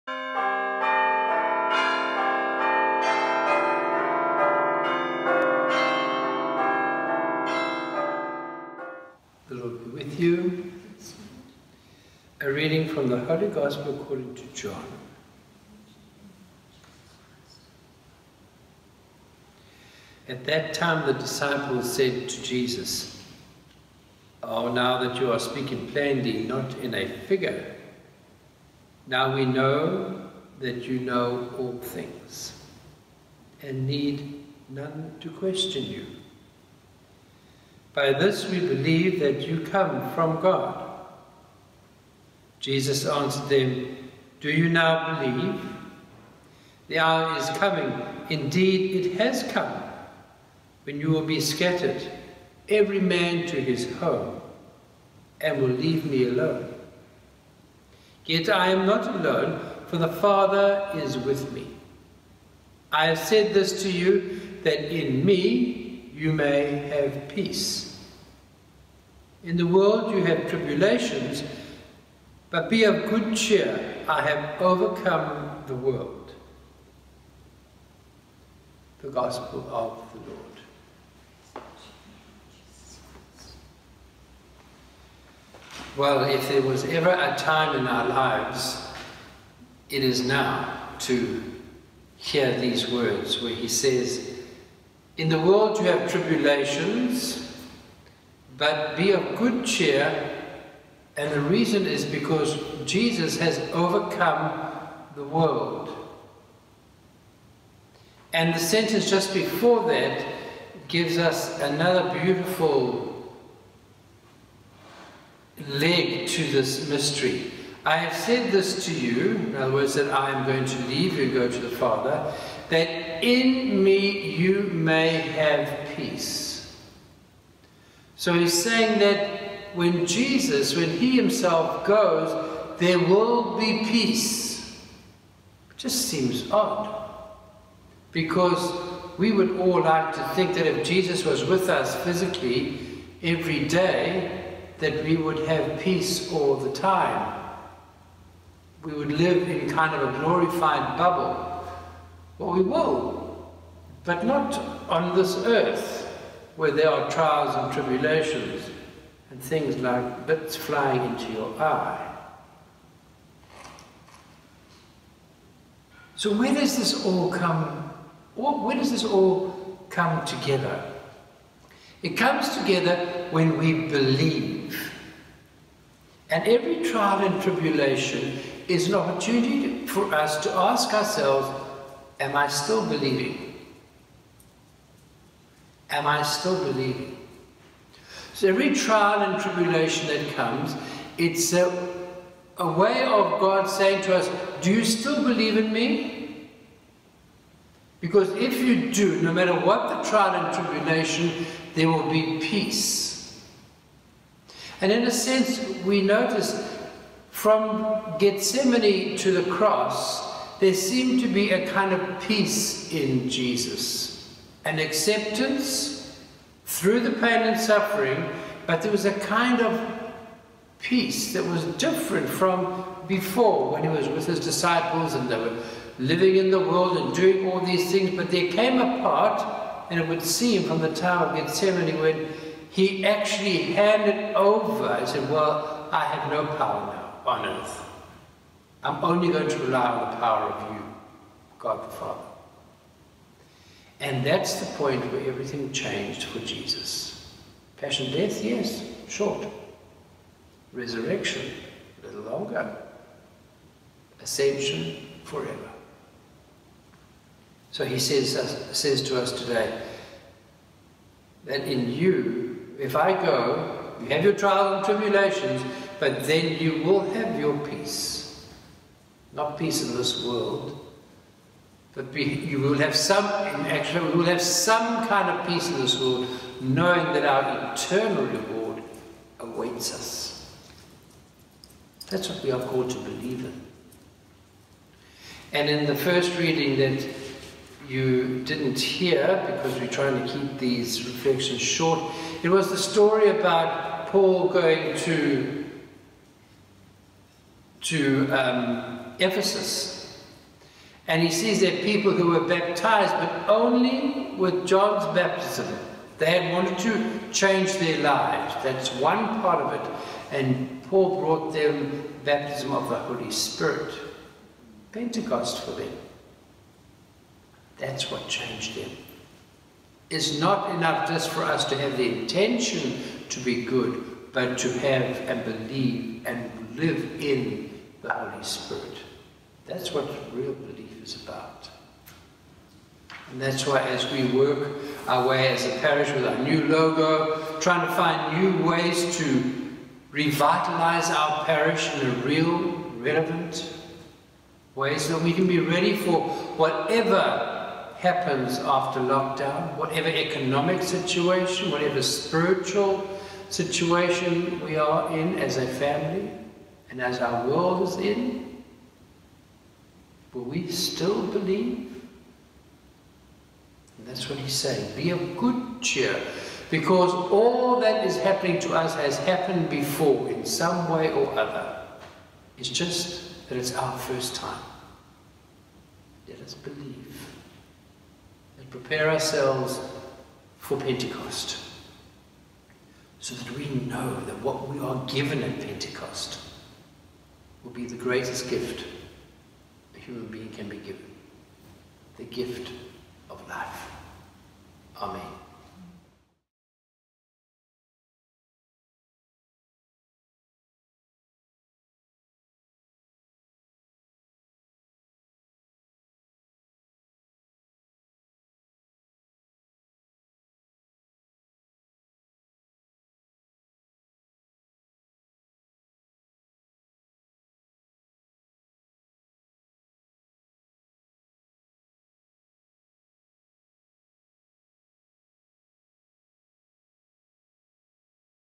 Reflection Week 7 Easter - Monday
Fr. Stephen's Morning Gospel Reading and Reflection
Holy Mass - Monday of the Seventh Week of Easter